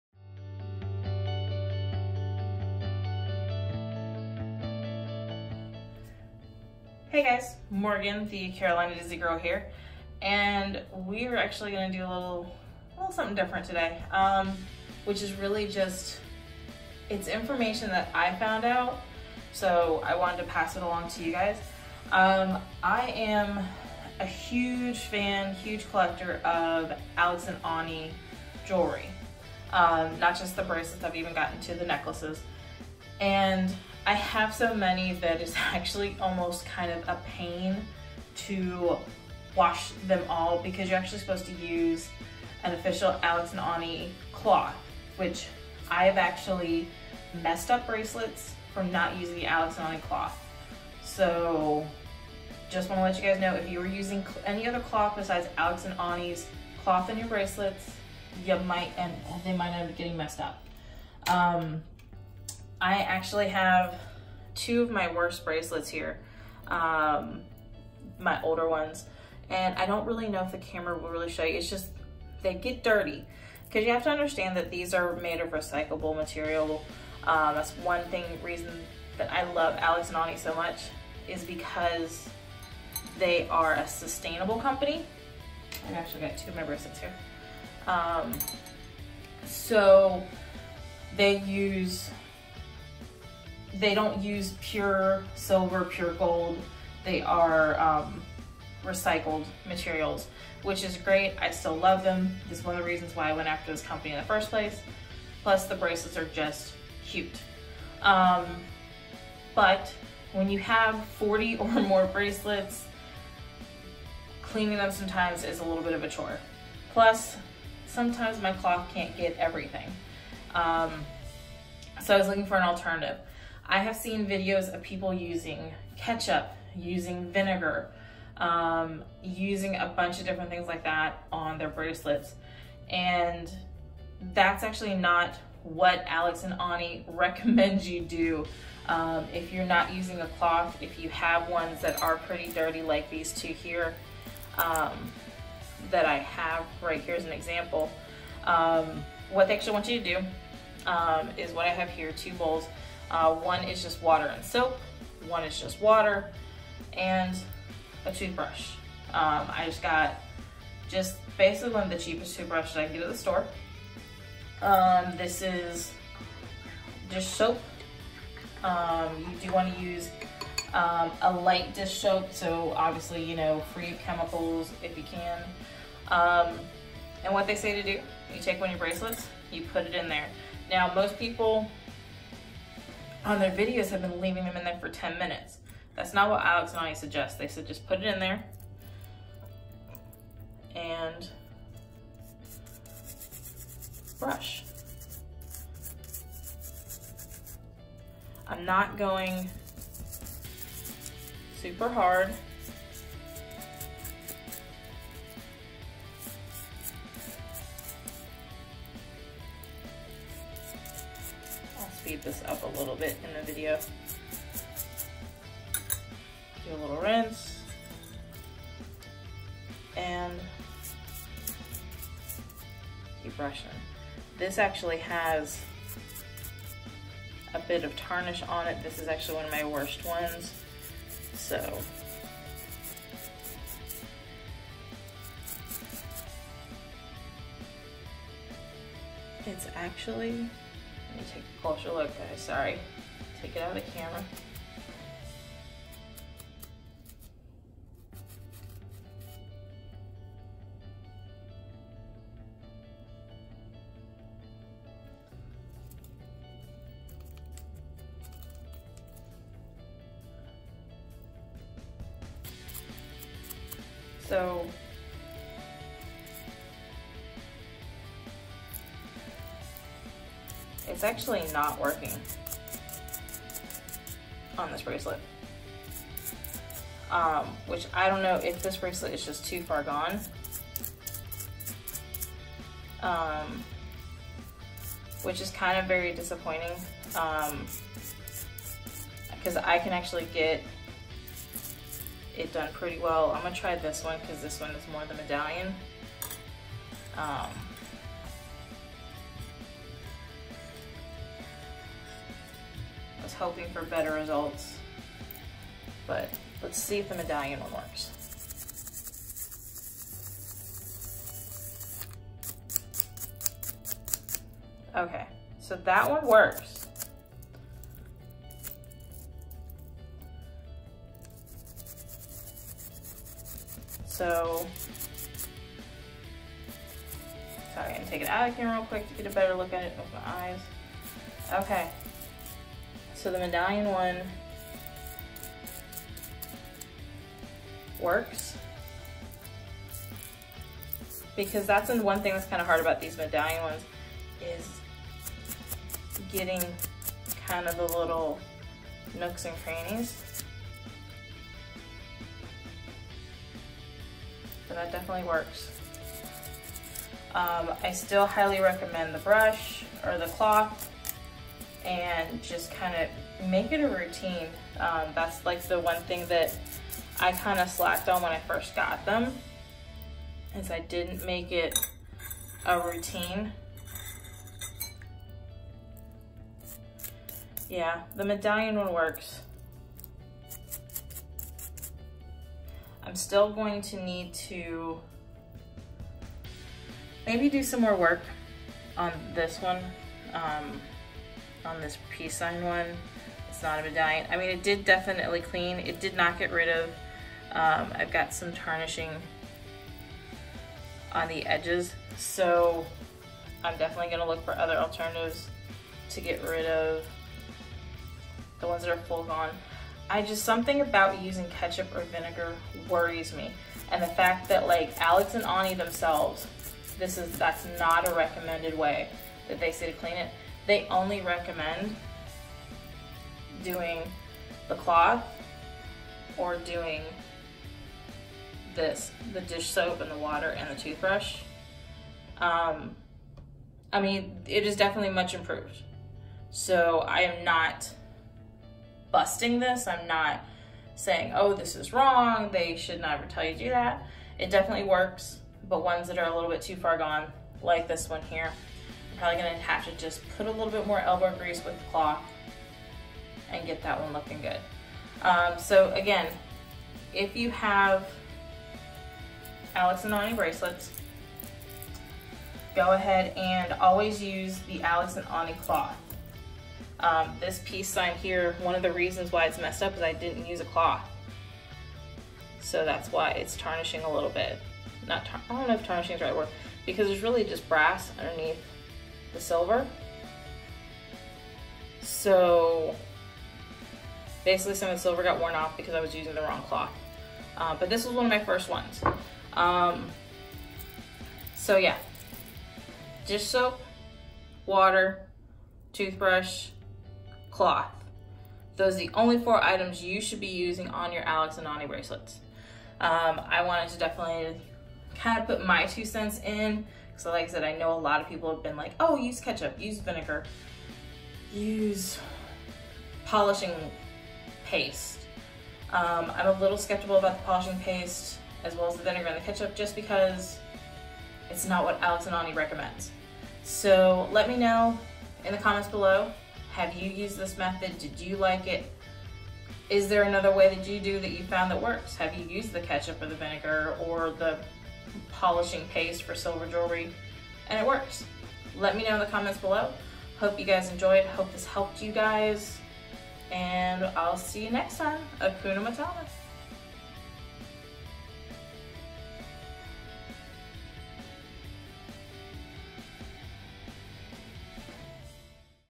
How To Clean Your Alex + Ani
I love Alex + Ani but hate always using the cloth. So I found an alternative way to clean your jewelry as recommended by the company! Check it out!